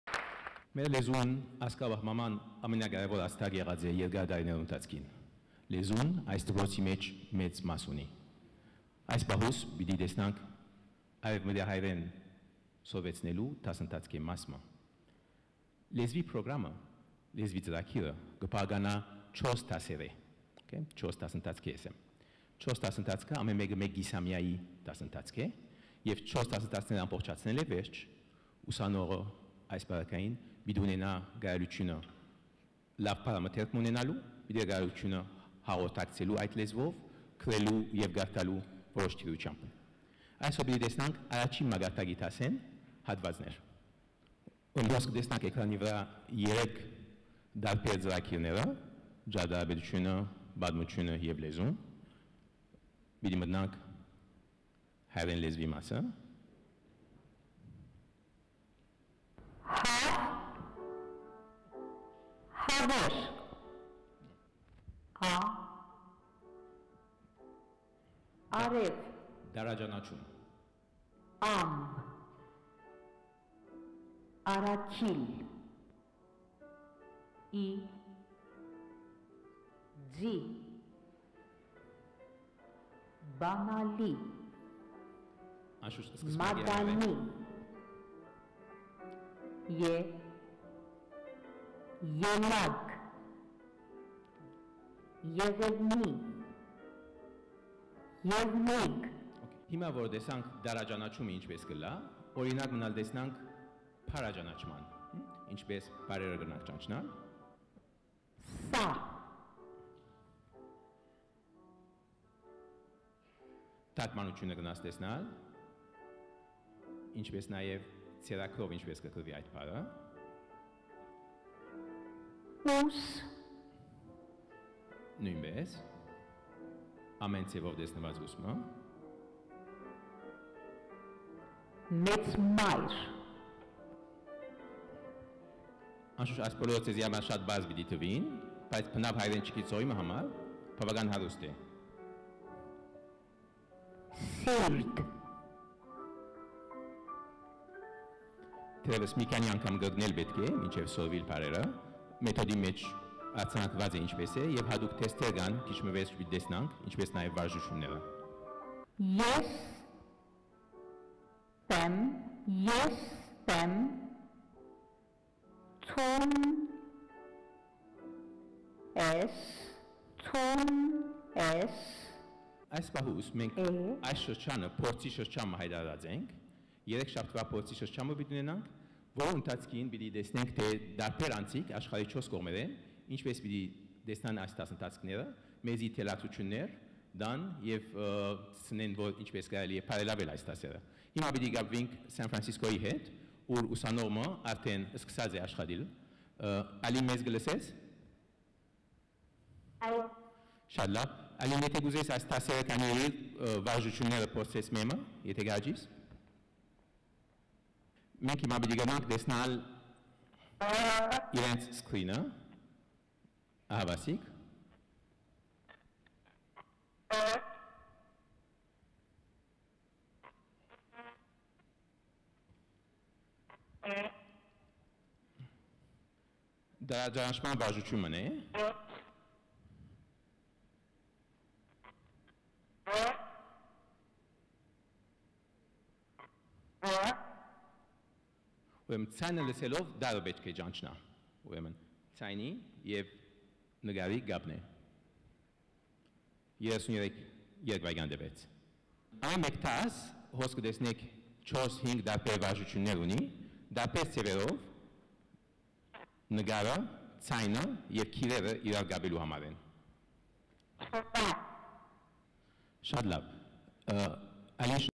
AGBU AVC launching ceremony / Beginners Western Armenian (in English)
Armenian Virtual college launching ceremony in Aram Khachaturian Concert Hall, Yerevan, Armenia